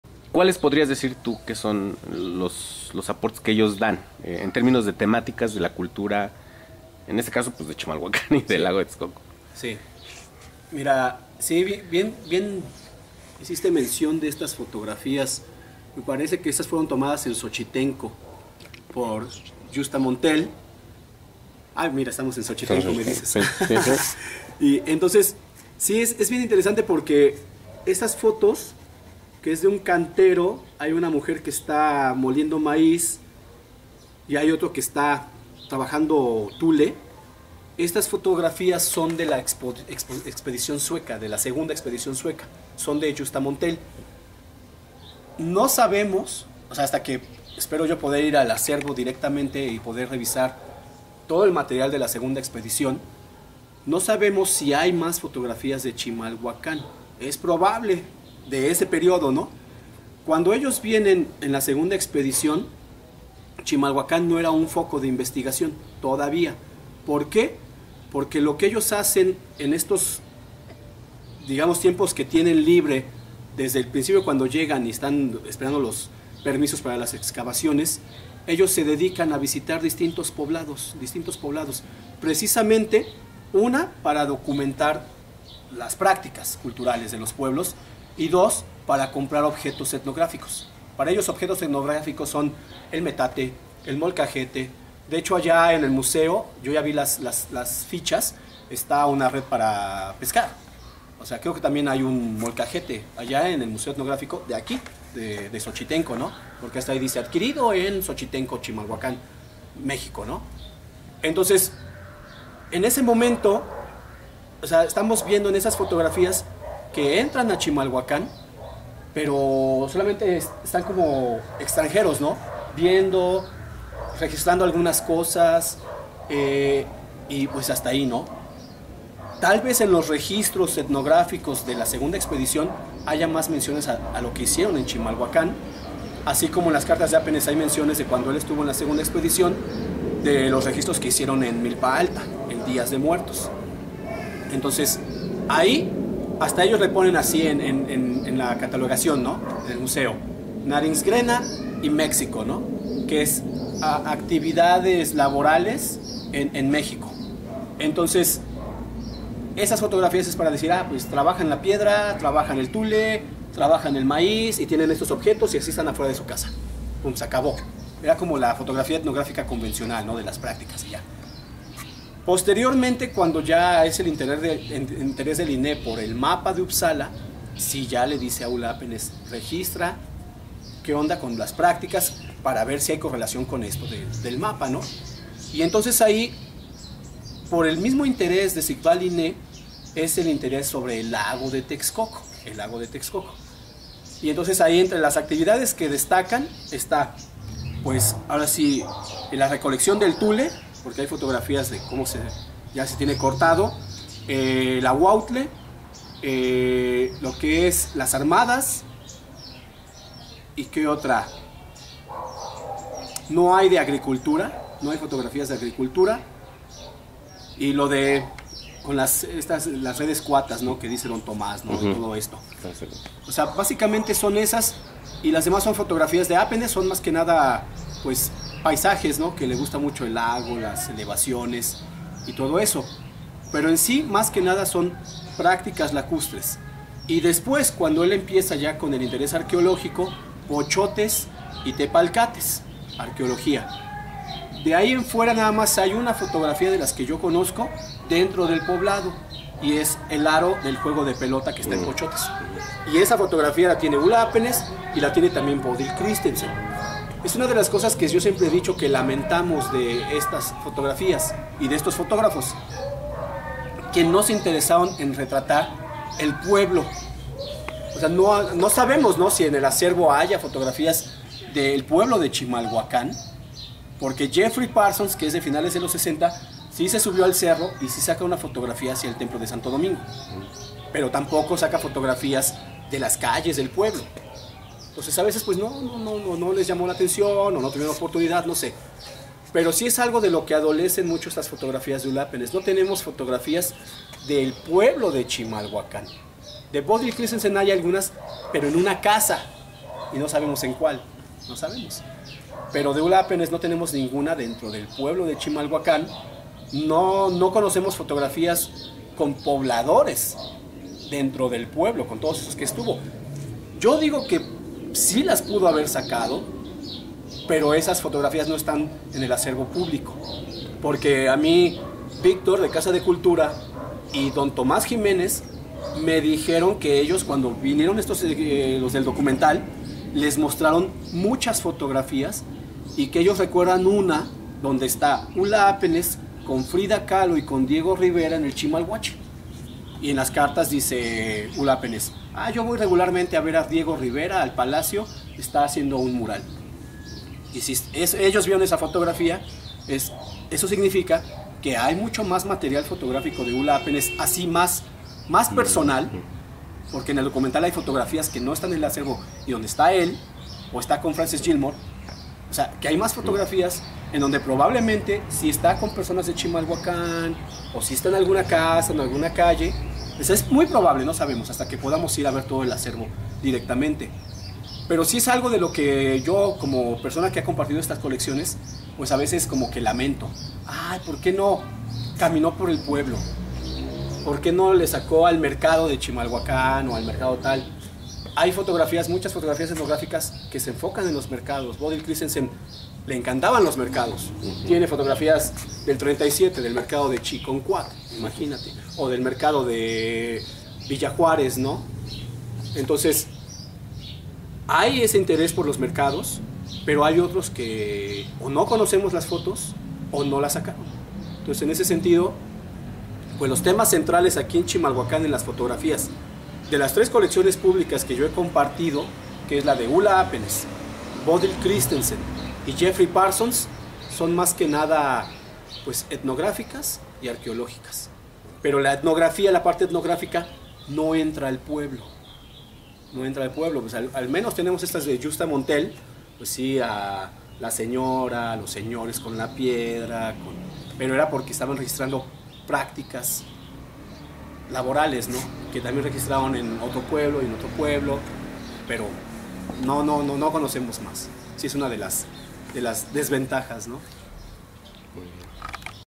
53. Chimalhuacán a finales de los 30´s: lineas de investigación
En esta ocasión entrevistamos a Martin Carrillo Ibáñez, alumno de maestría de la Escuela Nacional de Antropología e Historia, sobre el tema de los aportes escandinavos a la arqueología y etnografía de las actividades alrededor del lago de Texcoco en Chimalhuacán.
Sus aportes nos dan una excelente mirada al pasado de lo que fue el lago de Texcoco y las actividades lacustres de los pobladores de Chimalhuacán.
Se recomienda consultar su página en Facebook donde constantemente nos deja ver sus avances, pero sobre todo imágenes del Chimalhuacán de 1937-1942: